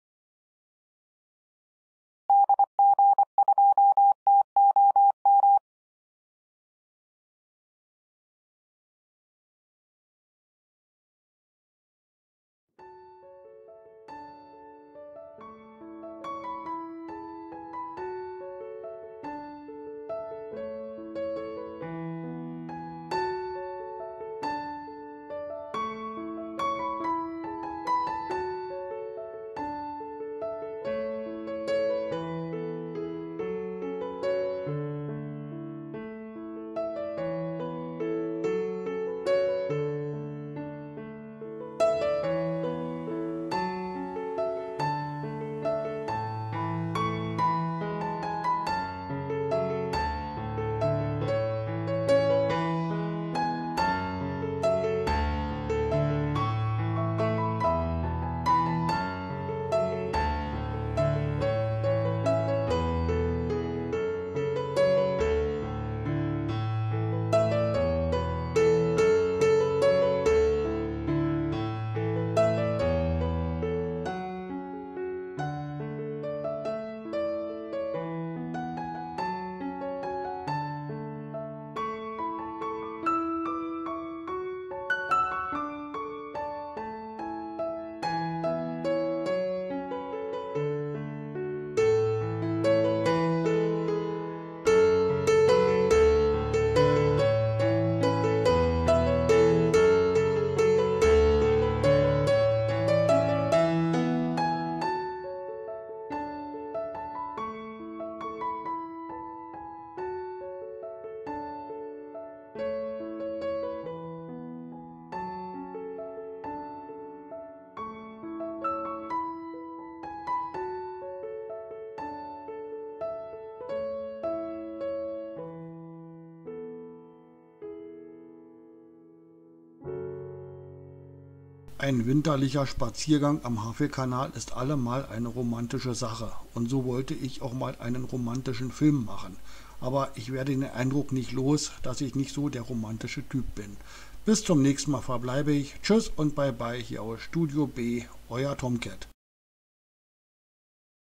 Wehranlage am Havelkanal
Ich wollte auch mal einen romantischen Film machen... also machte ich einen Winterspaziergang am Havelkanal...Ich werde aber den Eindruck nicht los, dass es mir nicht so richtig gelungen ist...
Die Wehranlage am Havelkanal ist aber sicher einen Film wert.